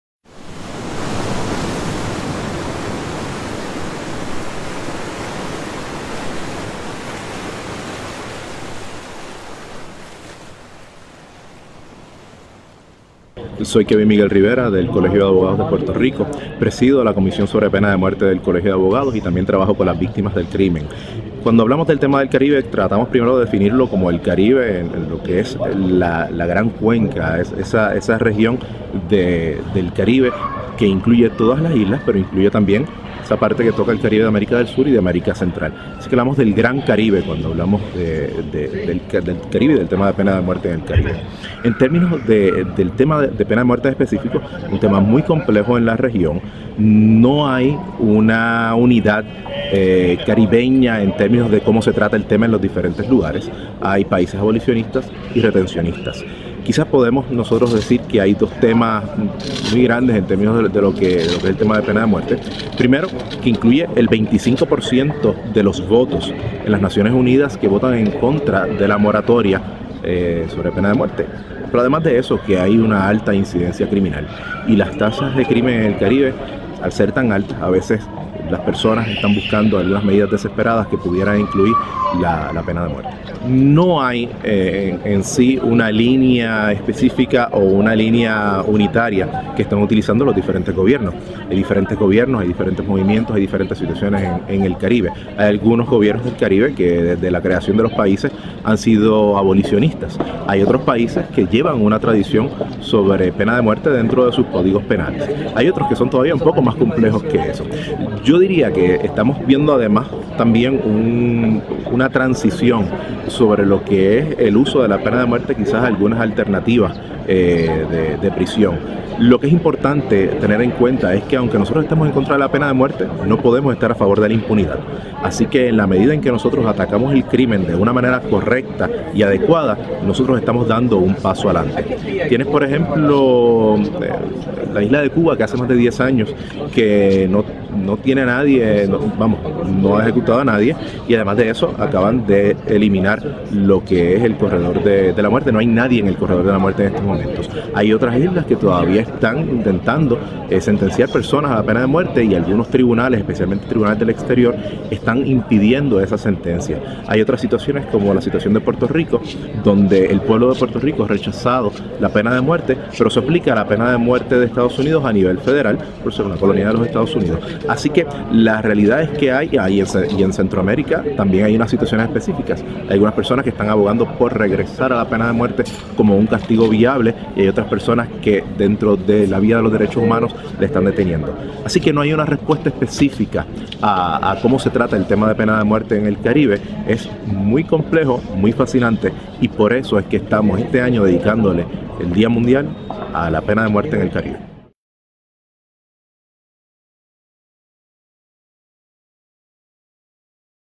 Kevin Rivera sobre la pena de muerte en el Caribe
Kevin Miguel Rivera, presidente de la comisión sobre la pena de muerte Colegio de Abogados de Puerto Rico, habla de la pena de muerte en el Gran Caribe en el 5 º Congreso Mundial contra la pena de muerte, Madrid, junio de 2013.
(video por la Coalición Mundial contra la Pena de Muerte)